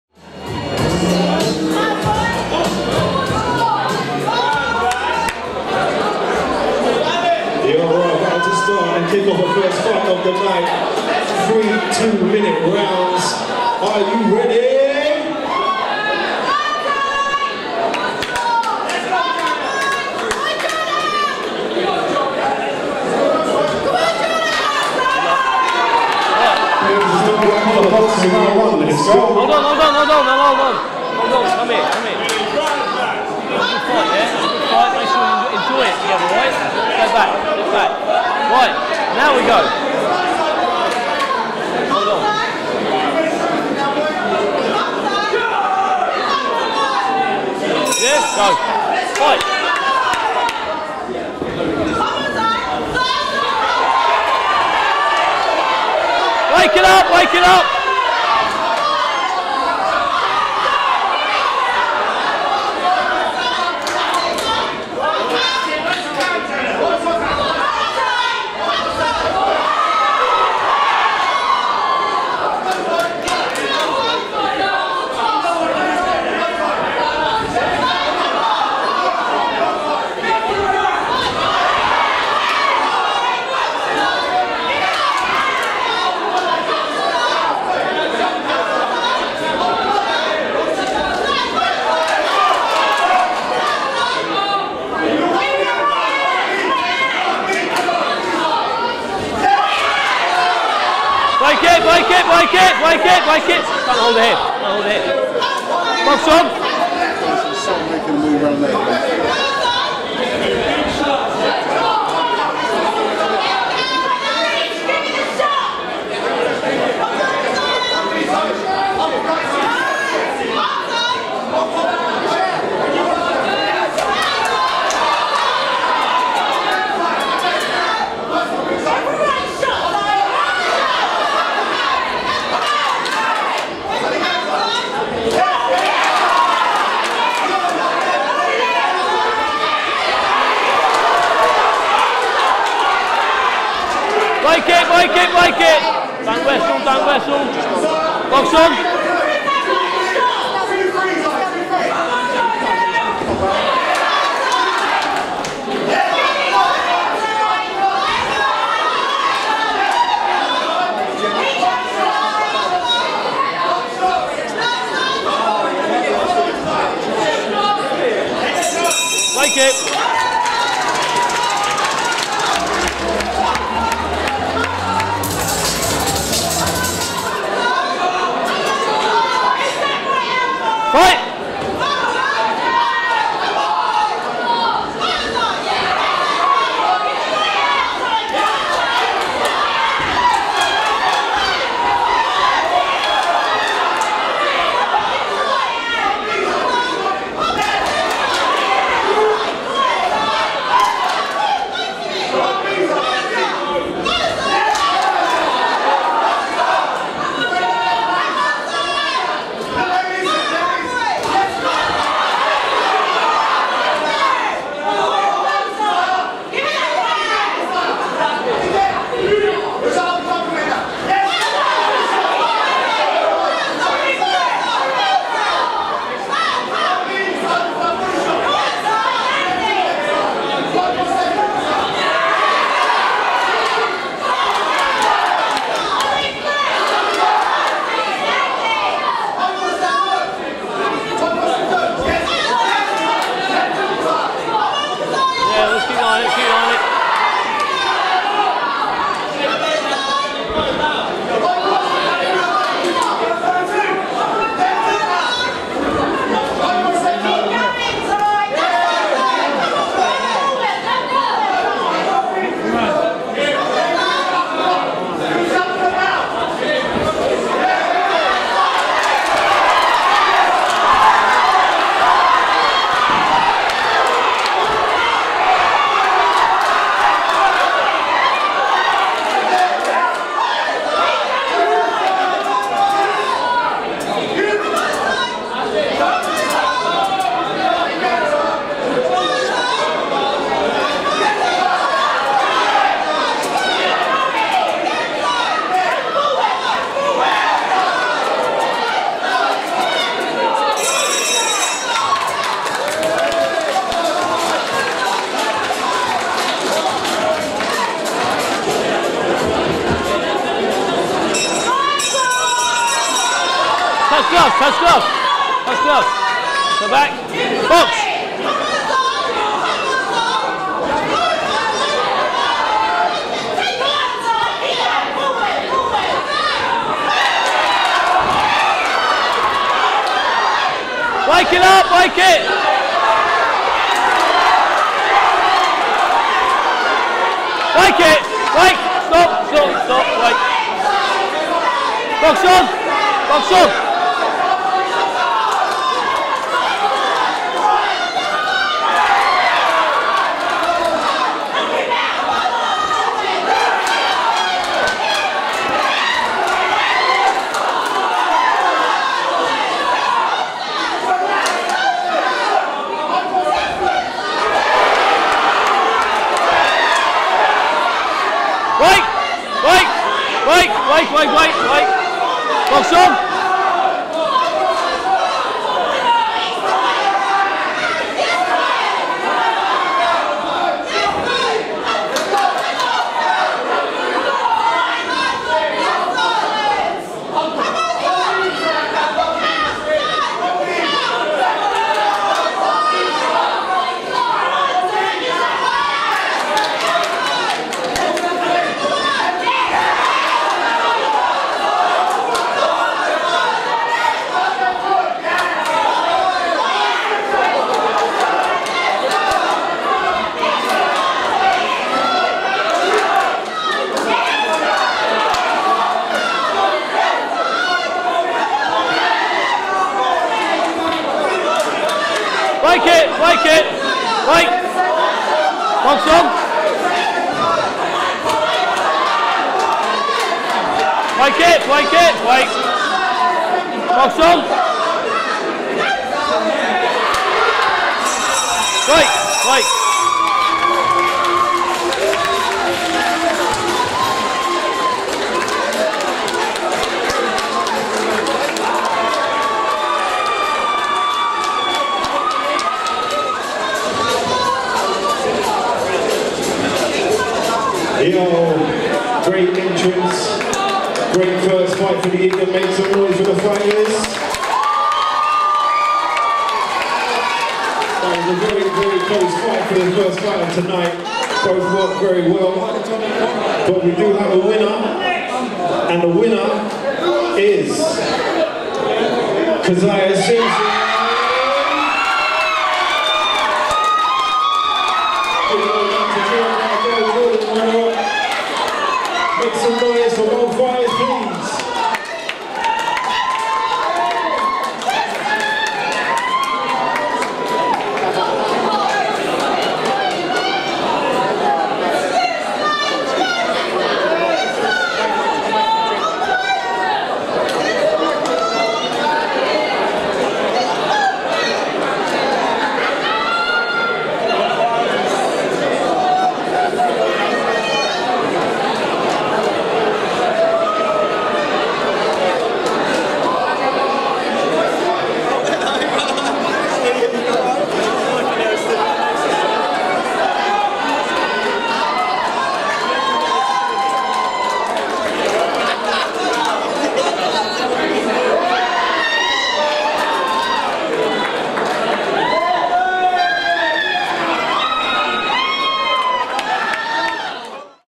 Basingstoke White Collar Boxing Bout 1 Jardel Jordan Vs Kaziah Simpson
Basingstoke White Collar Boxing
BWCB
10th December 2021
Jardel Jordan Vs Kaziah Simpson
Swindon Meca